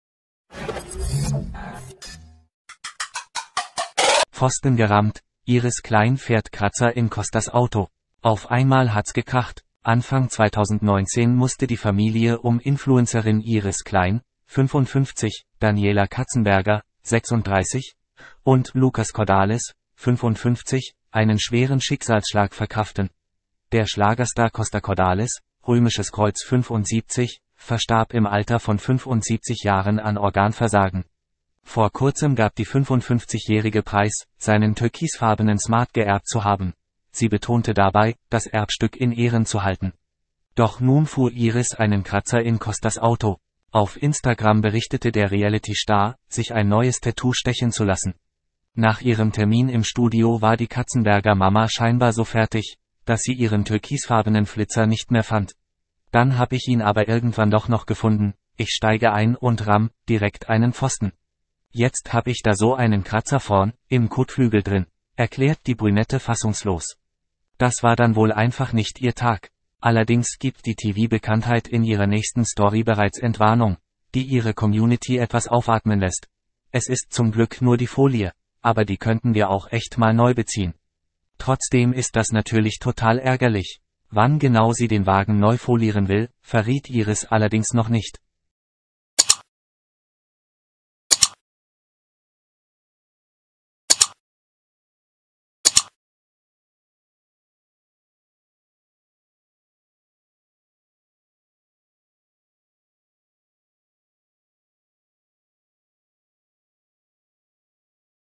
"Pfosten gerammt": Iris Klein fährt Kratzer in Costas Auto! | SH News German
Auf einmal hat's gekracht! Anfang 2019 musste die Familie um Influencerin Iris Klein (55), Daniela Katzenberger (36) und Lucas Cordalis (55) einen schweren Schicksalsschlag verkraften: Der Schlagerstar Costa Cordalis (✝75) verstarb im Alter von 75 Jahren an Organversagen. Vor Kurzem gab die 55-Jährige preis, seinen türkisfarbenen Smart geerbt zu haben – sie betonte dabei, das Erbstück in Ehren zu halten. Doch nun fuhr Iris einen Kratzer in Costas Auto!

Auf Instagram berichtete der Realitystar, sich ein neues Tattoo stechen zu lassen. Nach ihrem Termin im Studio war die Katzenberger-Mama scheinbar so fertig, dass sie ihren türkisfarbenen Flitzer nicht mehr fand. "Dann hab ich ihn aber irgendwann doch noch gefunden – ich steige ein und ramm' direkt einen Pfosten. Jetzt hab ich da so einen Kratzer vorn' im Kotflügel drin", erklärt die Brünette fassungslos.